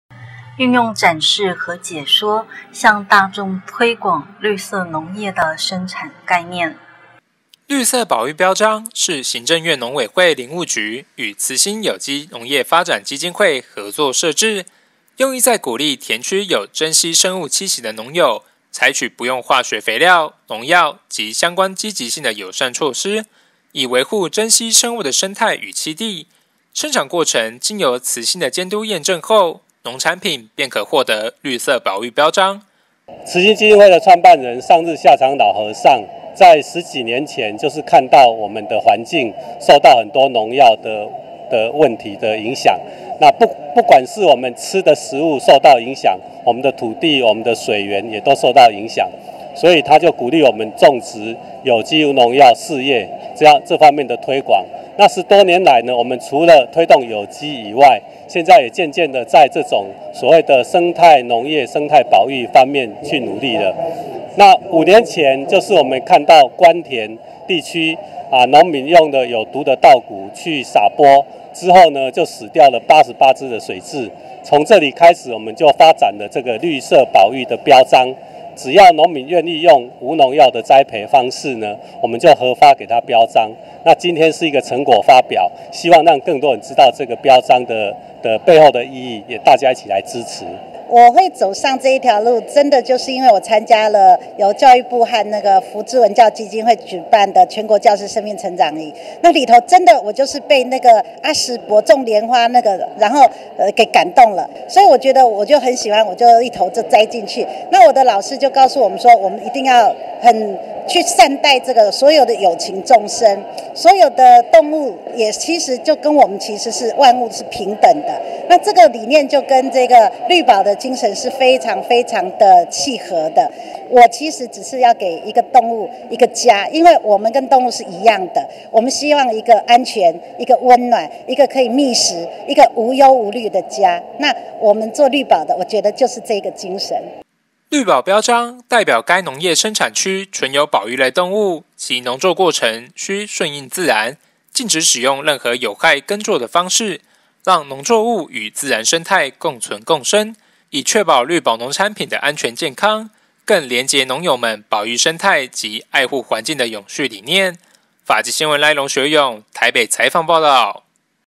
法界新聞》20141210自然生態 共存共生
法界新聞》
受訪者：慈心有機農業發展基金會
「田裡有腳印市集」讓民眾透過市集活動，從理念教育中觸發感動，選擇友善環境的生活方式。
運用展示及解說、蔬食品嚐等多元形式，向社會大眾推廣綠色保育農業生產的理念。